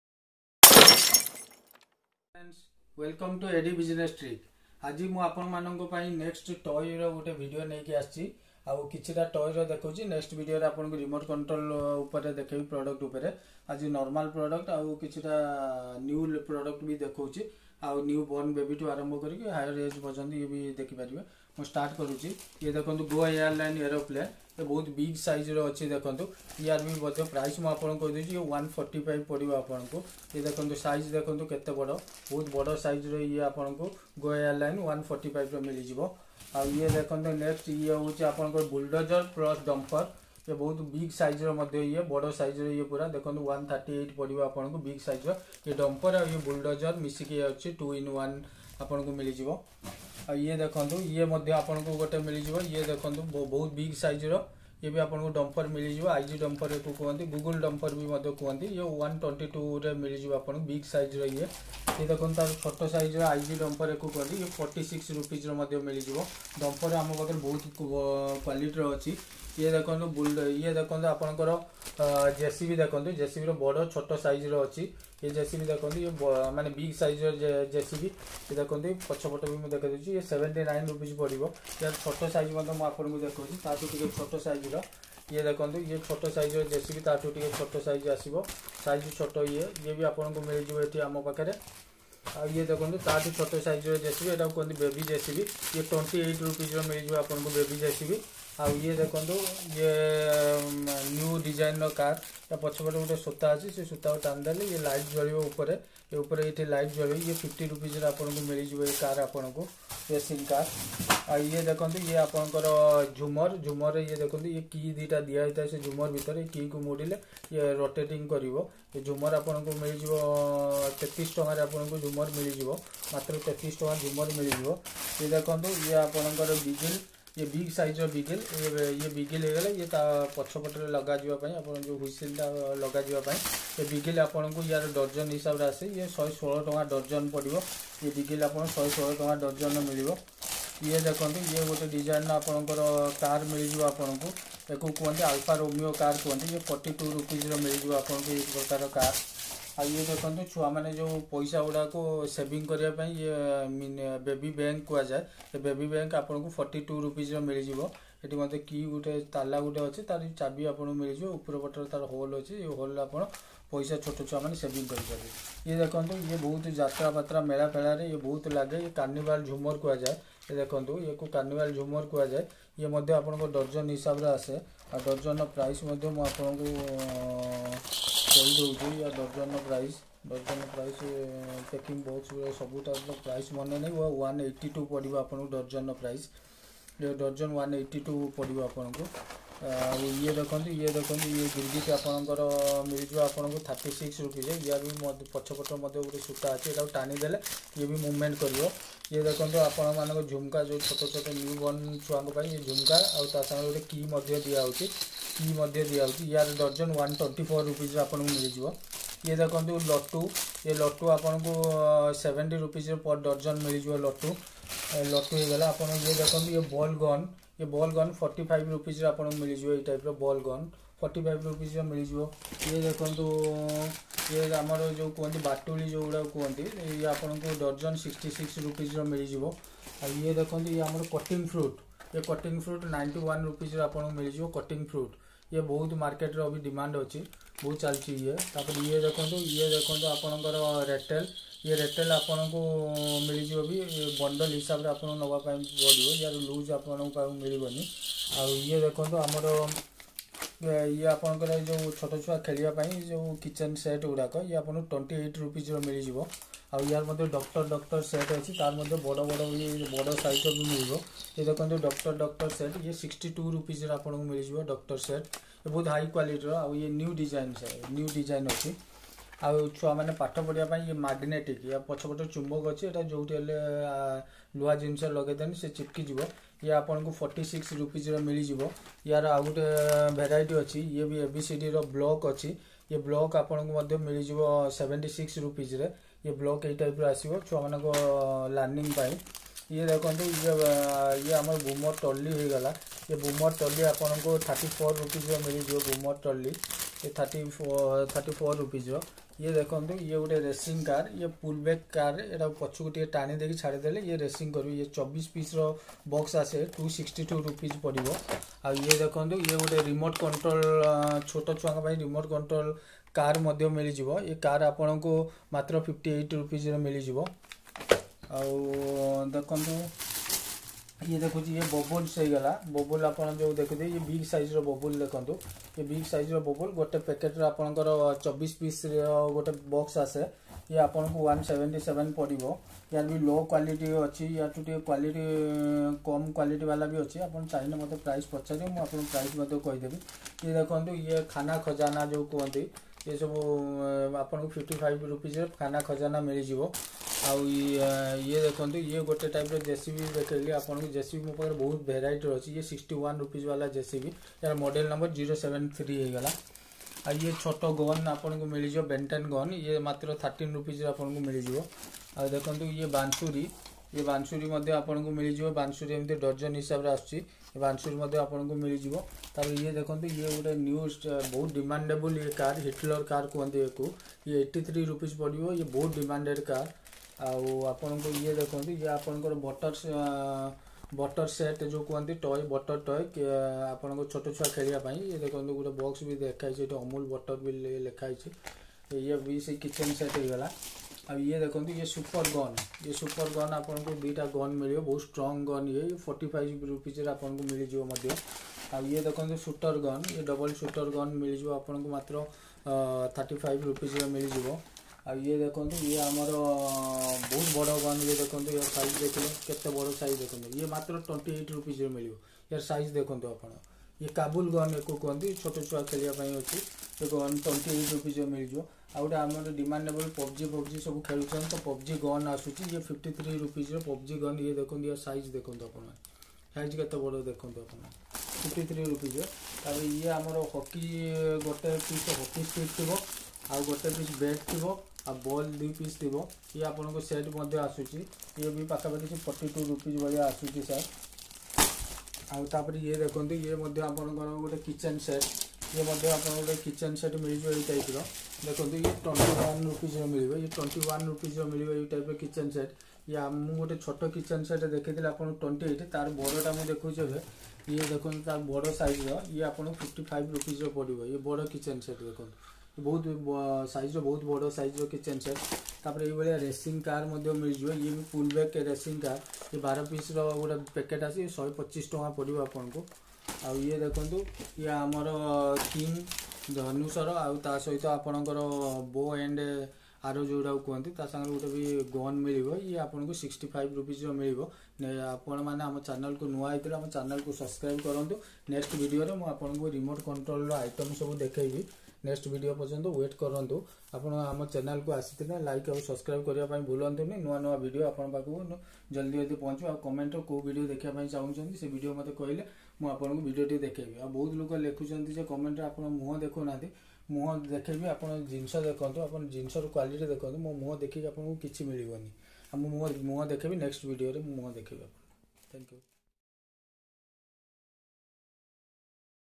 Cheapest toy market in Odisha | Unique toys wholesale shop helicaptor,dron,Cars,bike,JCB,doll,teddy
Deals All types Kids toys, teddy,doll and balloon etc.